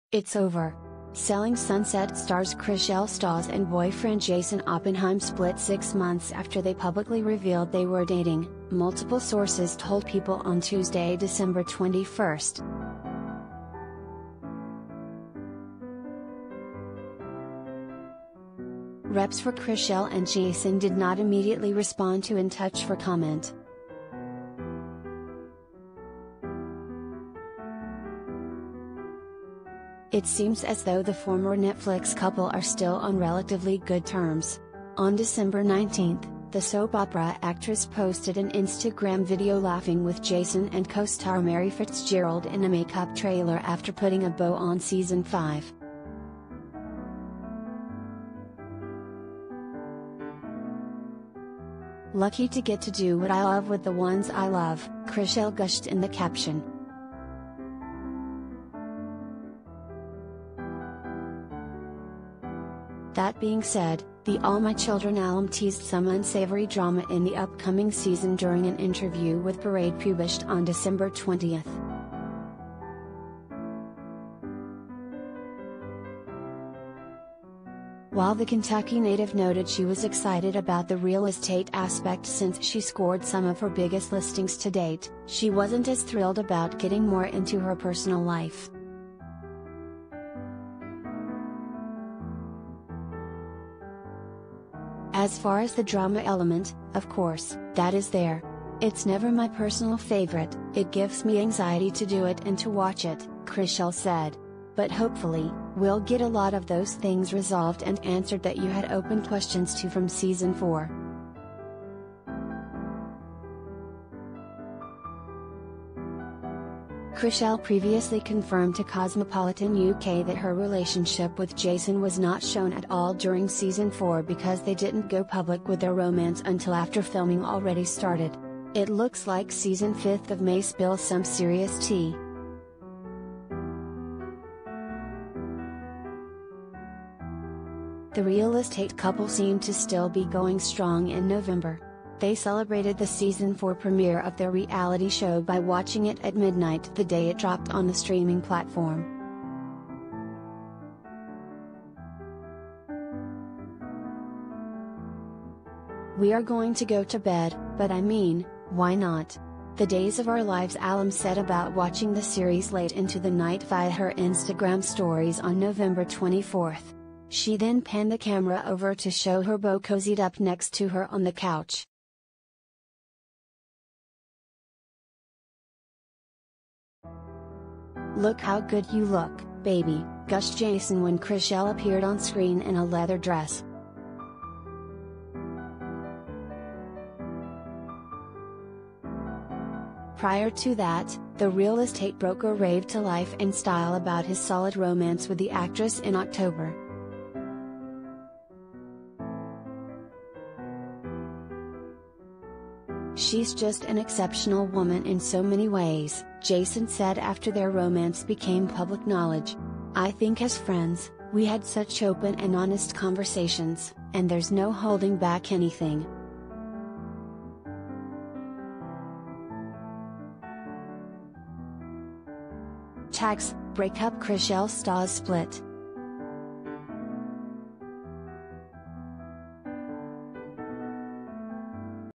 It's Over! Selling Sunset's Chrishell Stause and Jason Oppenheim Split
It’s over! Selling Sunset stars Chrishell Stause and boyfriend Jason Oppenheim split six months after they publicly revealed they were dating, multiple sources told People on Tuesday, December 21.
Reps for Chrishell and Jason did not immediately respond to In Touch for comment.
It seems as thoug